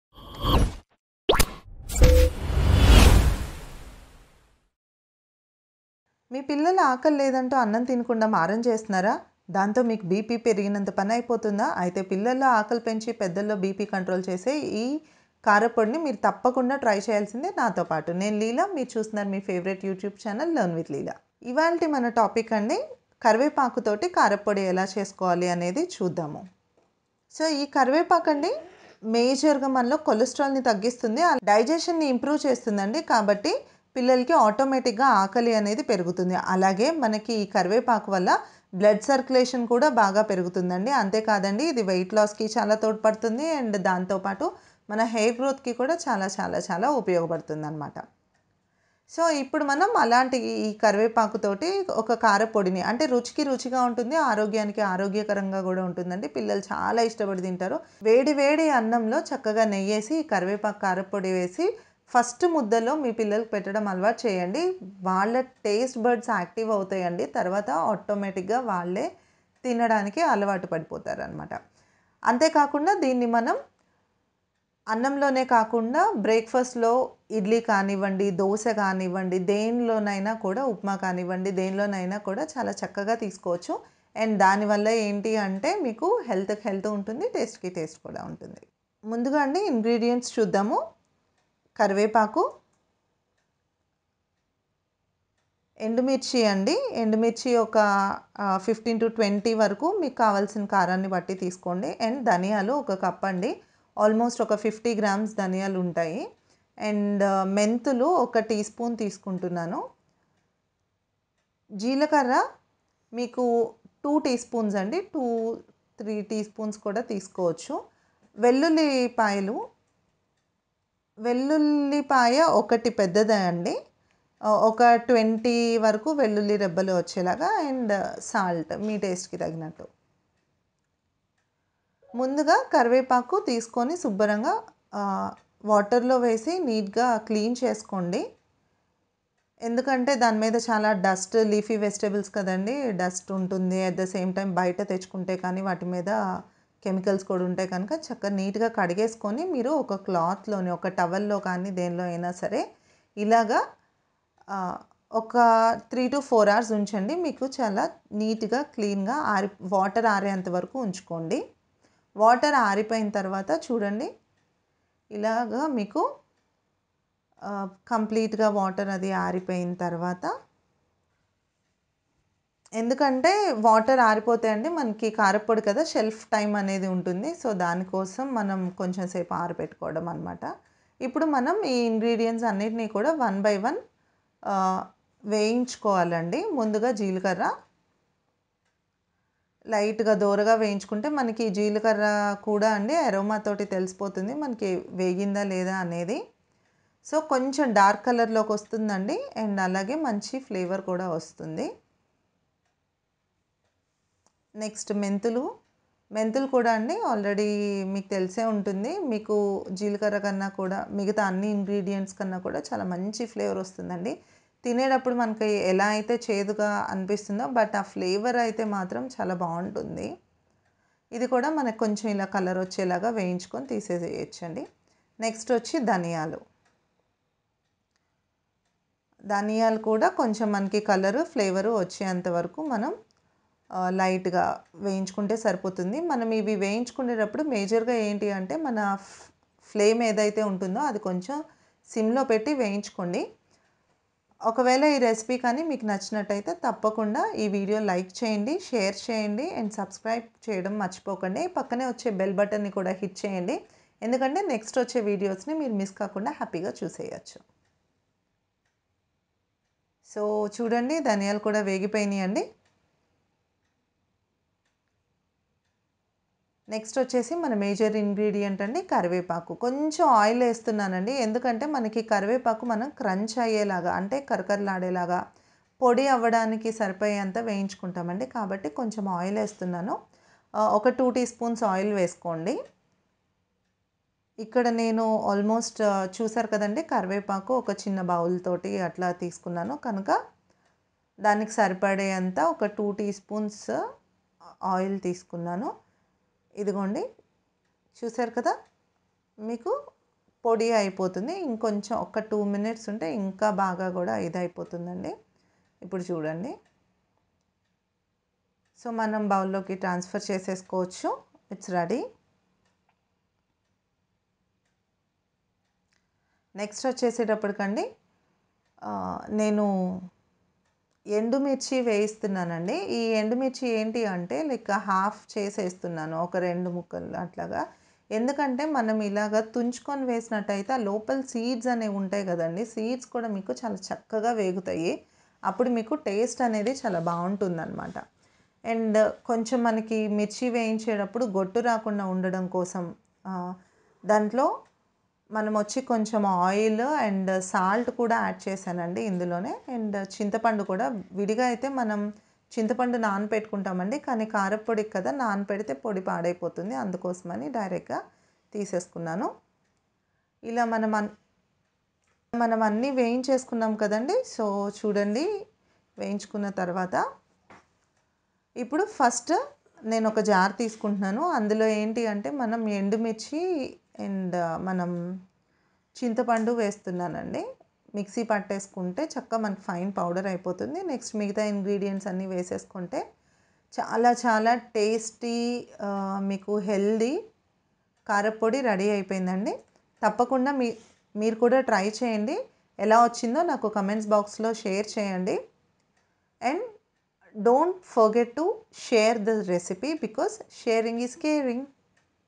TASTY & HEALTHY Boosts kids Appetite&Reduce BP in adults పిల్లల్లోఆకలి పెంచిపెద్దల్లోBP తగ్గిస్తుంది
A wonderful Reciepe that enhances your taste buds. improves digestion and increses Appetite of you kids. also improves blood circulation and regulates BP. can be used both with RICE & EVERYDAY BREAKFAST
Ingredients:
Curry leaves
Redchillies - 20 - 25(Based on your taste and type of chillies)
Coriander seeds - 50gms
cumin - 2tsp
Methi - 1 tsp
Garlic - 1 full(big size)/20 cloves (big size)